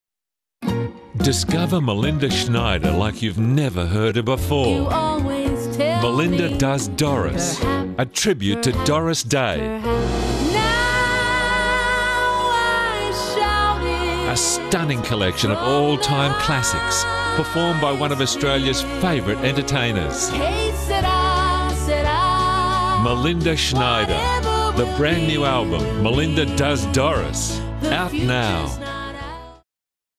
Melinda Schneider - a tribute to Doris Day
After forging a dazzling country music career that has won her 6 Golden Guitars, and stunning audiences on Dancing with the Stars, Australian favourite, Melinda Schneider steps back in time to pay homage to one of her musical heroes, Doris Day.